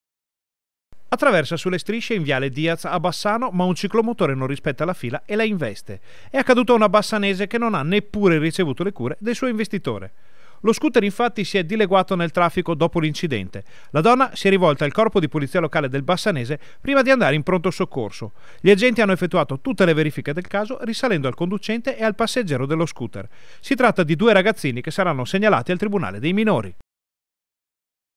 INVESTE UNA DONNA CON LO SCOOTER E SCAPPA: DENUNCIATO | 16/02/2021
TG Bassano del Grappa 16/02/2021 - BASSANO DEL GRAPPA - Due ragazzini di Bassano sono stati denunciati per essersi allontanati dal luogo di un incidente stradale || Passa sulle strisce in viale Diaz a Bassano, ma un ciclomotore non rispetta la fila e la investe: è accaduto a una bassanese, che non ha neppure ricevuto le cure del suo investitore. Lo scooter, infatti, si è dileguato nel traffico dopo l’incidente. La donna si è rivolta al corpo di polizia locale del bassanese prima di andare in pronto soccorso. Gli agenti hanno effettuato tutte le verifiche del caso, risalendo al conducente e al passeggero dello scooter. Si tratta di due ragazzini, che saranno segnalati al tribunale dei minori. (Servizio di Ferdinando Garavello) - Segui Rete Veneta anche sul digitale terrestre! Visita il sito